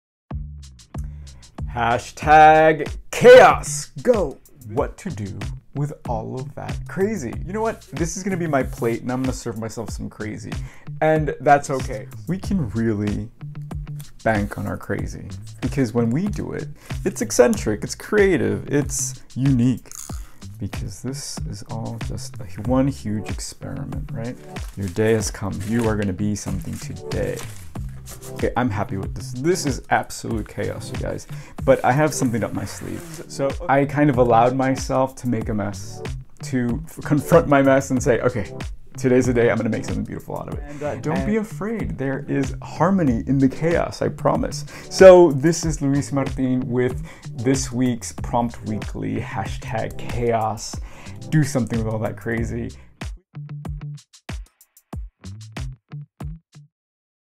Prompt Weekly Teaser Trailer
Join me in this pointedly cathartic session. In this class we do good with what we have and invite happy accidents by using our hoards of existing materials. Use the mess that you might have in folders, boxes or even storage units across town and find harmony in the chaos.

Step into the studio as a safe space to explore ideas, have conversation with your hands and make a collage in realtime. Every session is a masterclass in flow and participating in the low risk and high reward of creating something from nothing.

Go to CollageDream.com to purchase this session now for $5 during its first week and $10 after.
There is much adventure to had in this collage dream!